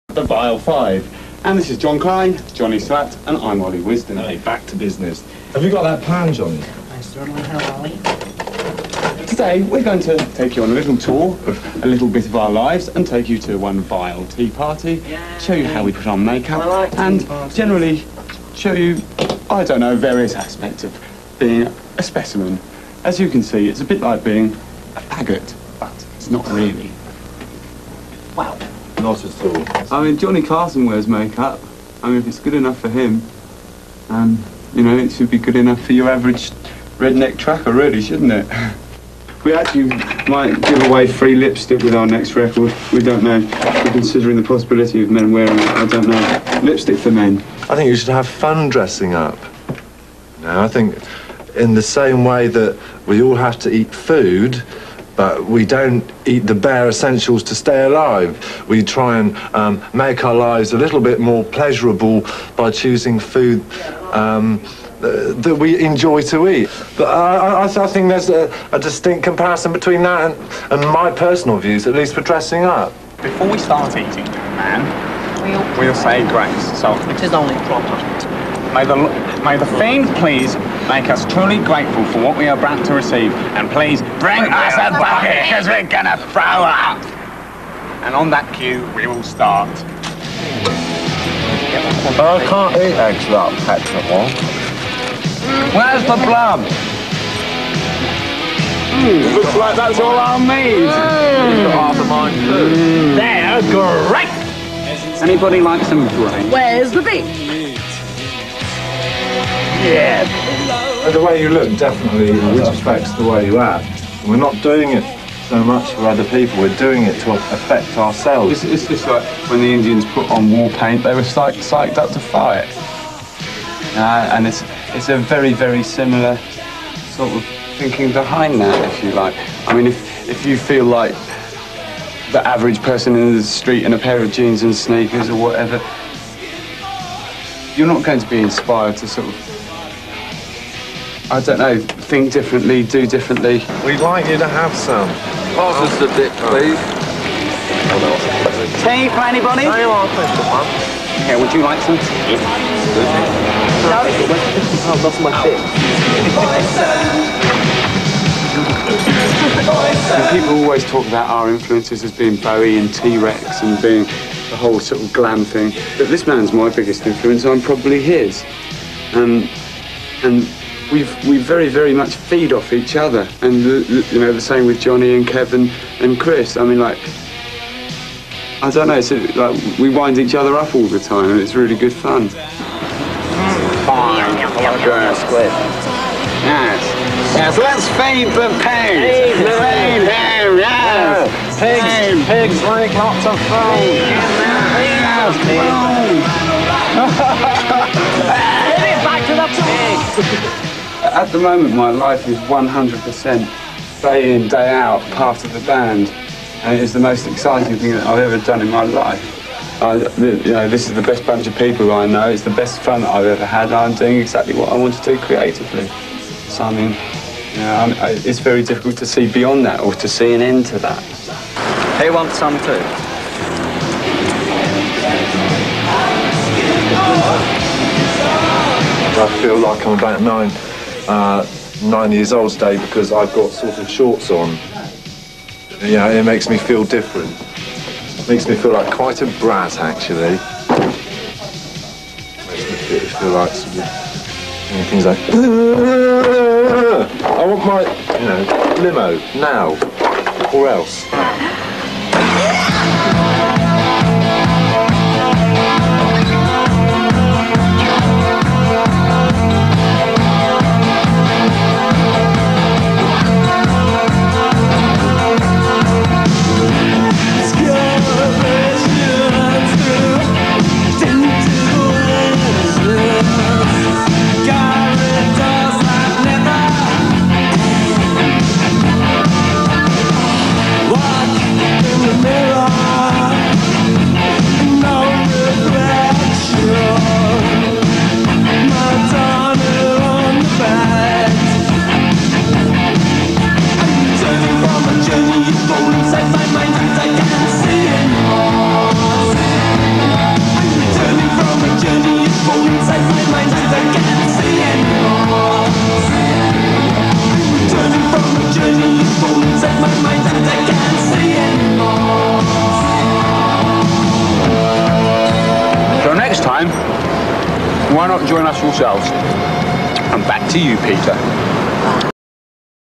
Specimen Interview and Lunch
Interview and Lunch with Specimen circa 1983